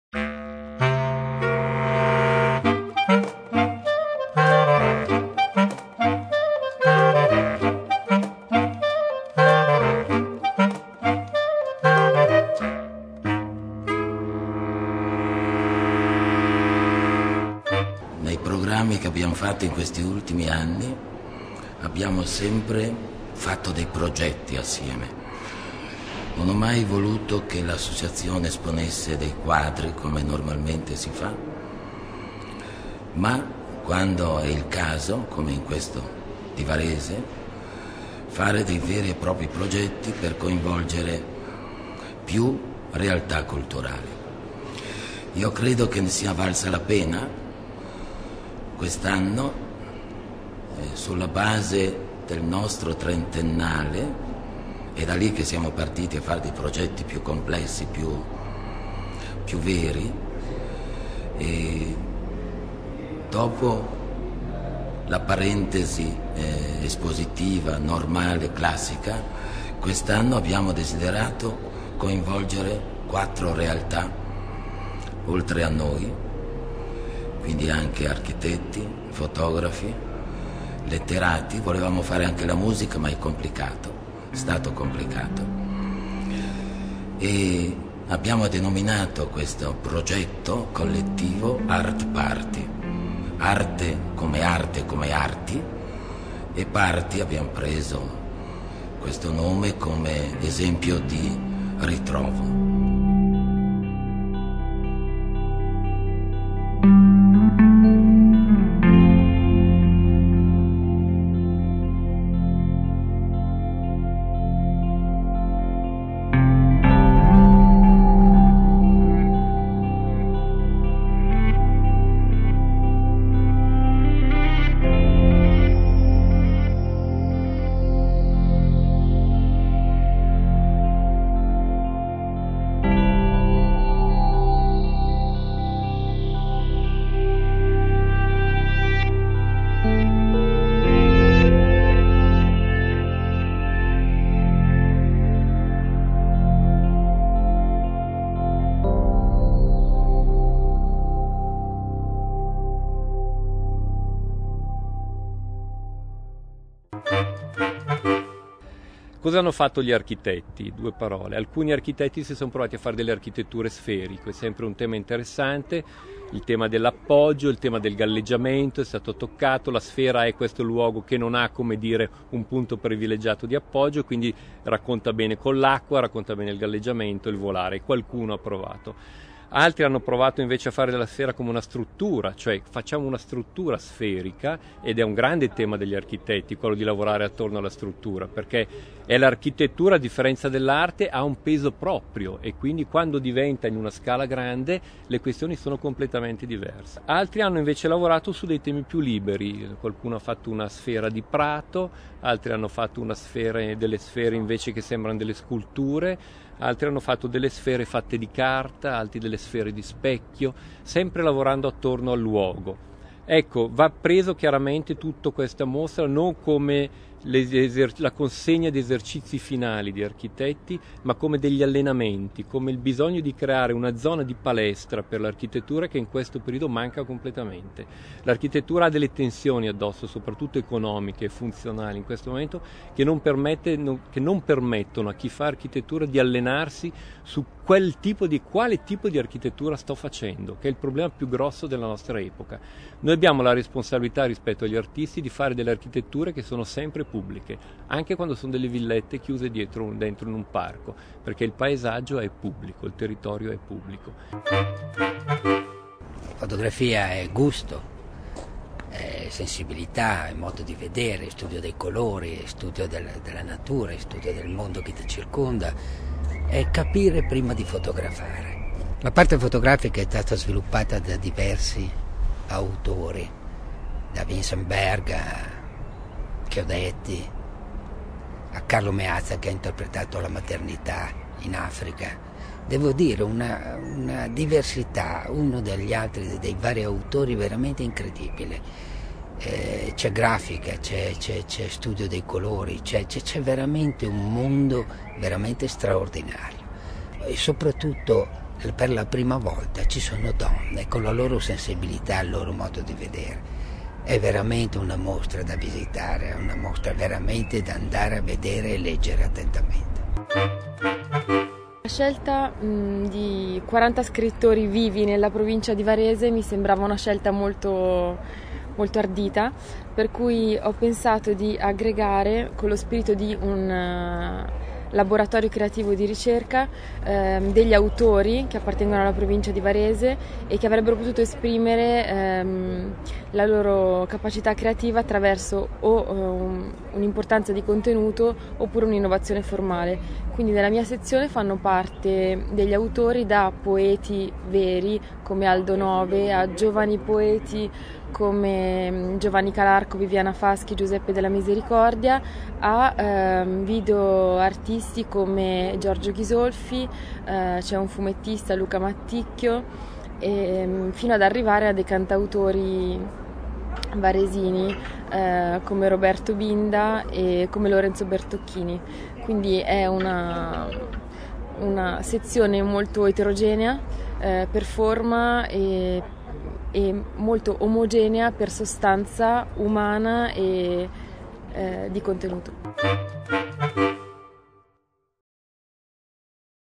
Artparty - Sferica 2010
Il Comune di Varese, in collaborazione con l’Associazione Liberi Artisti della Provincia di Varese, fondata nel 1977 e oggi composta da 72 artisti, tra cui pittori e scultori, promuove un progetto espositivo denominato “ARTPARTY 2010” tra artisti, architetti, fotografi e letterati della nostra Provincia. Questo titolo, ci si augura che venga utilizzato ogni anno o ogni biennio, con autori e progetti comuni, con temi culturali diversi.

Proponendo questo progetto, Marcello Morandini ha voluto per la prima volta coinvolgere più forze culturali attive sul territorio e sottolineare in questo modo lo spirito e il valore progettuale che dovrebbero avere le future edizioni di “ARTPARTY”.

La mostra ha come tema la “sfera” intesa come luogo, dimensione, materiale o spirituale, fisica, ecc. secondo l’interpretazione che ogni partecipante vorrà dare.

La sfera è un gioco rimbalzante equiperimetrico difficilmente fermo, è la forma ideale per ogni corpo che prema dal suo interno, è delimitata dalle orbite degli elettroni, è un frutto nel cui interno la geometria frattale ne costruisce le parti, è una membrana e un vuoto, è un pieno non contenuto, è la visione comune di spazio.

La sfera è il luogo della Madre Terra, rappresenta l’ordine costituito dalle forze gravitazionali in miliardi di anni dopo l’ipotesi della grande esplosione primordiale, è un luogo geometrico in cui tutti i punti che ne definiscono la superficie hanno una comunione di equidistanza dal centro, è un racconto che intreccia infinite storie circolari, è una poesia che ci permette di ritrovare la nostra unità nella molteplicità infinita dei punti, è un’immagine che ridefinisce uno spazio bidimensionale esplodendolo in uno sferico, è un’architettura che non ha fondamenta, non appartiene a un luogo, è in costante movimento… o tutto il contrario di ciò.

Museo d’Arte Moderna e Contemporanea - Castello di Masnago - Varese